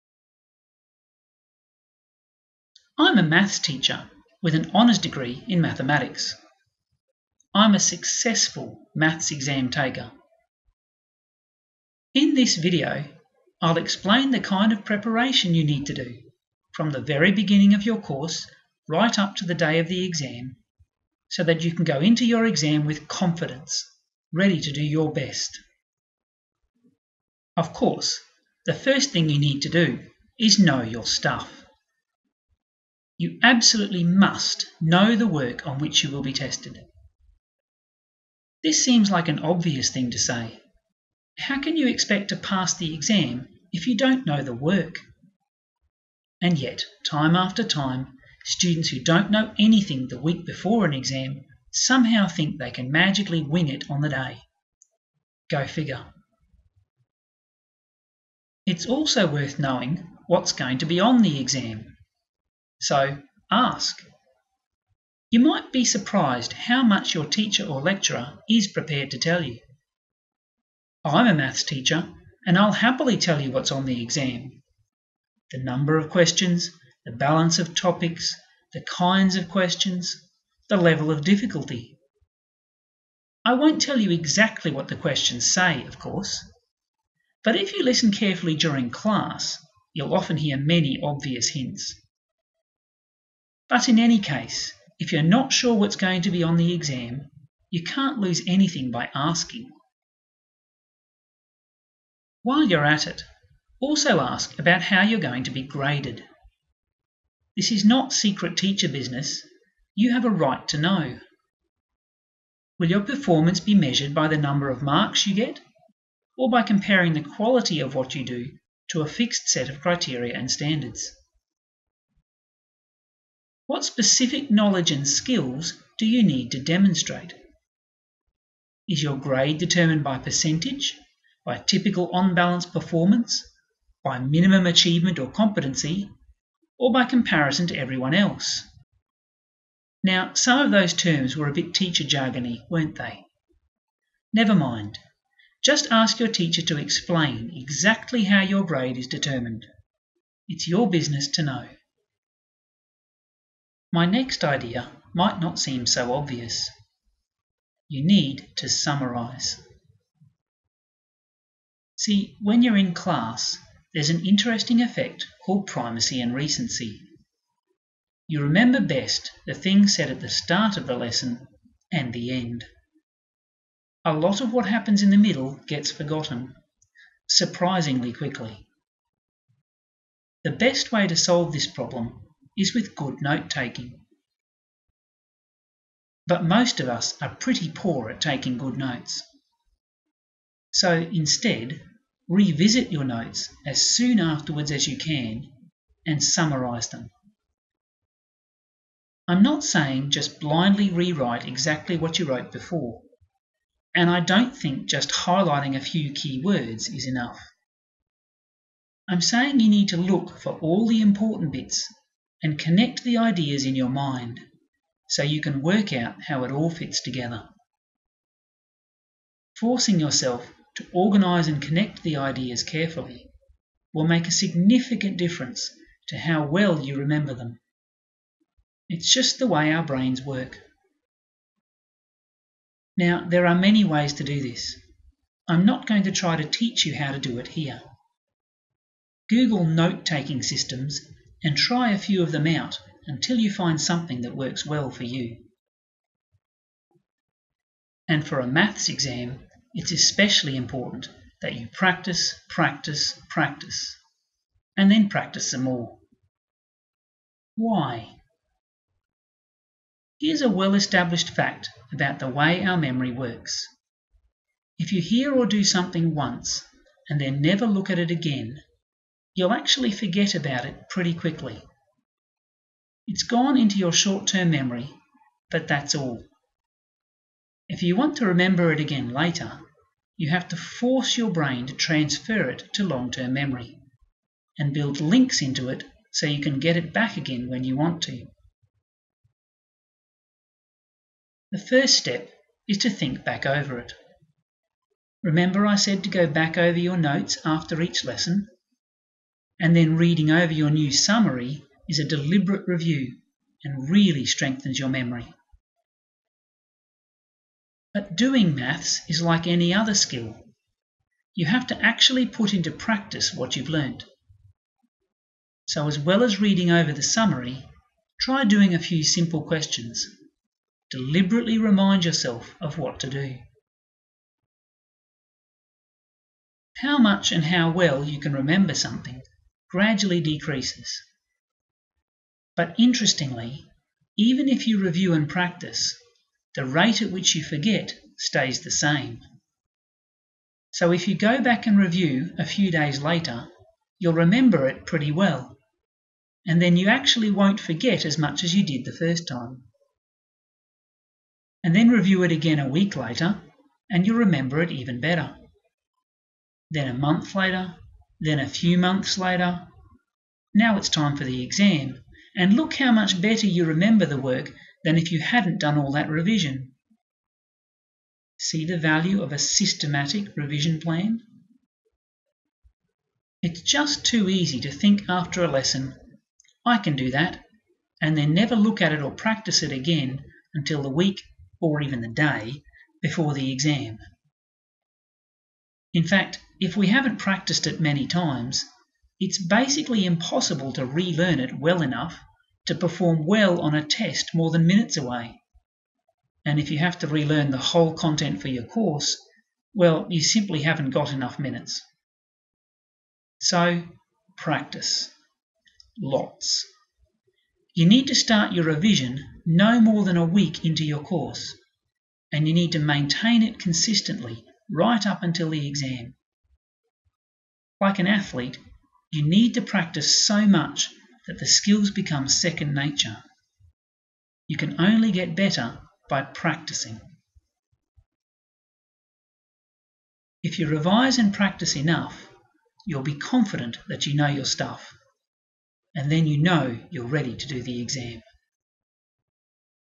How to prepare for a maths exam - and succeed!
Exam technique for a maths exam, part 1 - How to prepare

Important things to know and find out
The value of good notes, summaries and revision

Of course, what I say here is also applicable to non-maths exams!

* Bonus: There's a good summary page of various note-taking systems